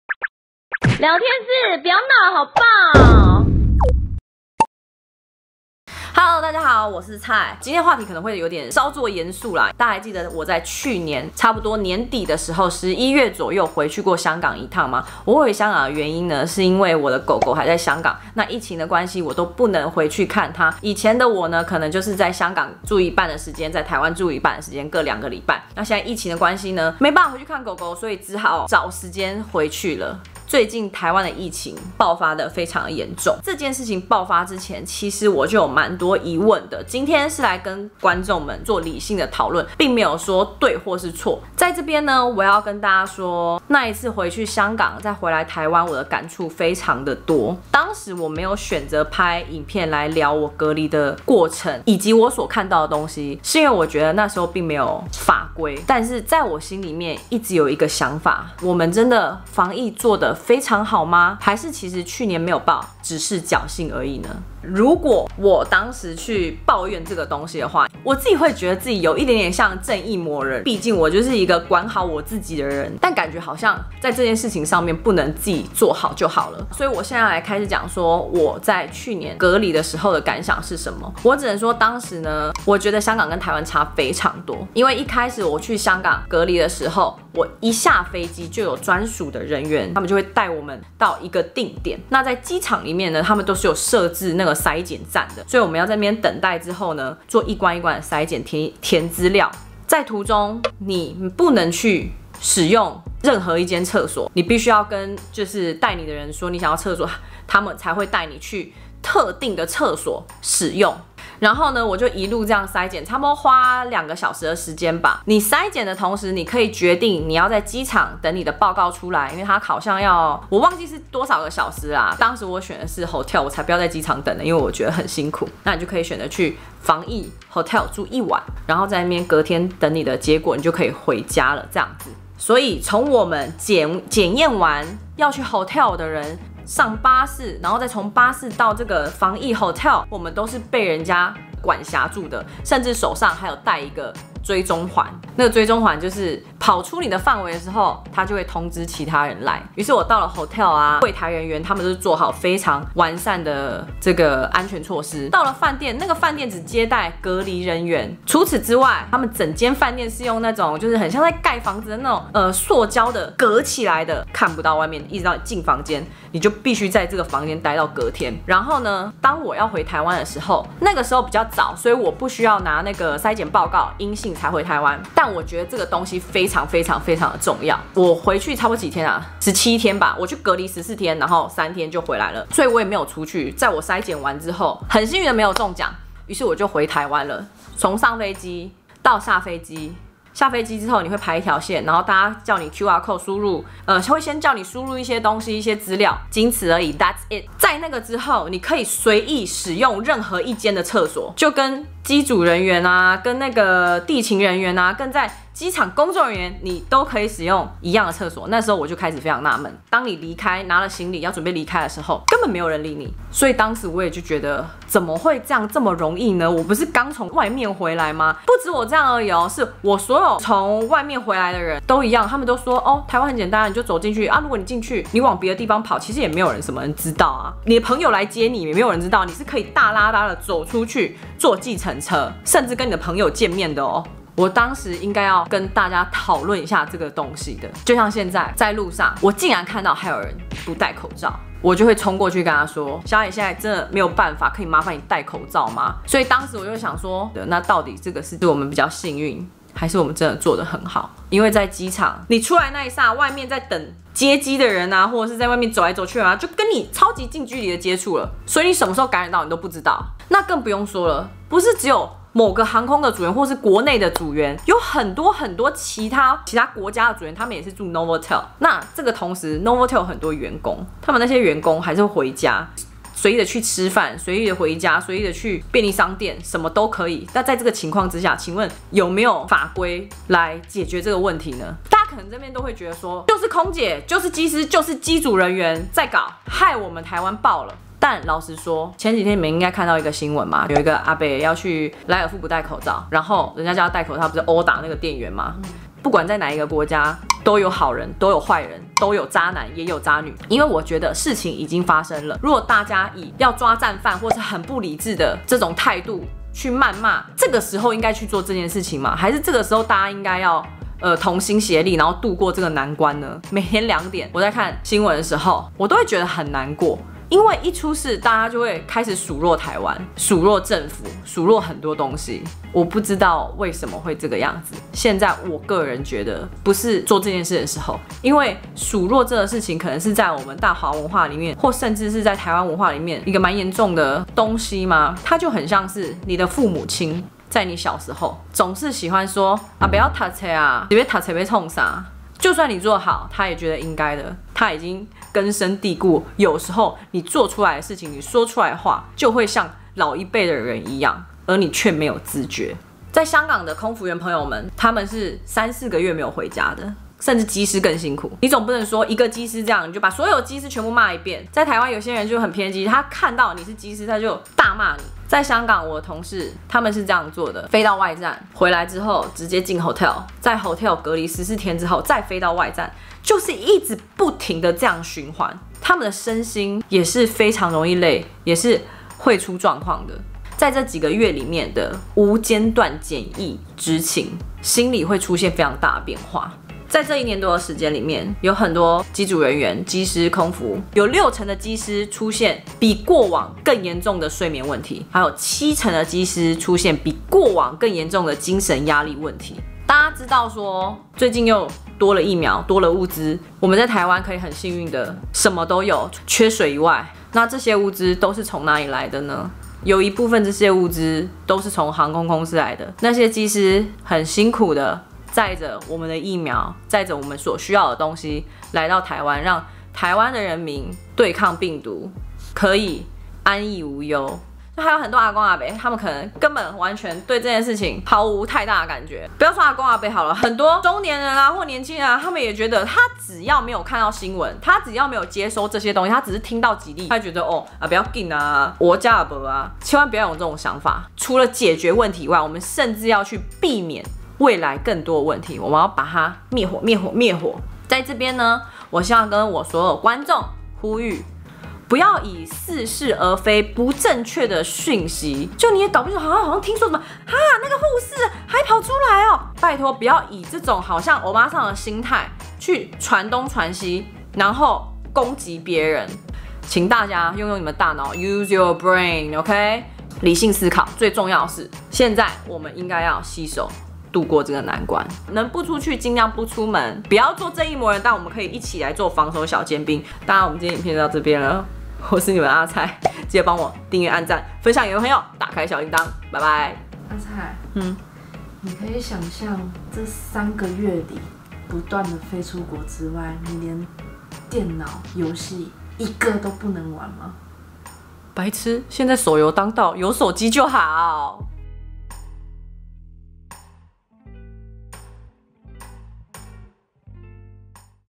機場防疫破口 全台三級警戒 去年台灣沒爆發 是幸運？還是僥倖？【天菜小姐嚴肅議題】
這次的疫情來的很突然
不管是媒體報導也好
或是坊間流傳也罷
希望大家都能攜手渡過這次的風波